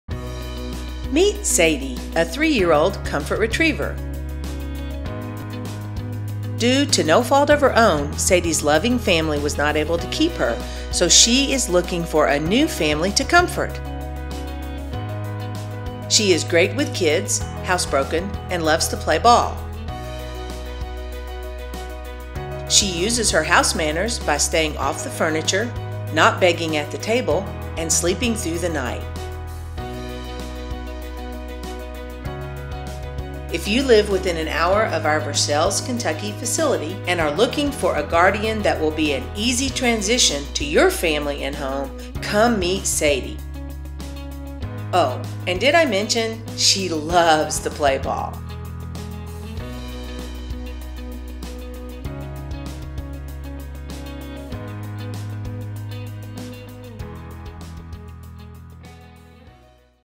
Sadie (JQ4): Guardian Snapshot (HOMED)
Meet Sadie, a 3-year-old Comfort Retriever! Due to no fault of her own,  Sadie's loving family was not able to keep her, so she is looking for a new family to comfort! She is great with kids, housebroken, and loves to play ball. She uses her house manners by staying off the furniture, not begging at the table, and sleeping through the night.

If you live within one hour of our facility in Versailles, KY, and are looking for a Guardian that will be an easy transition to your family and home, meet Sadie (JQ4)! Oh, and did I mention, she loves to play ball?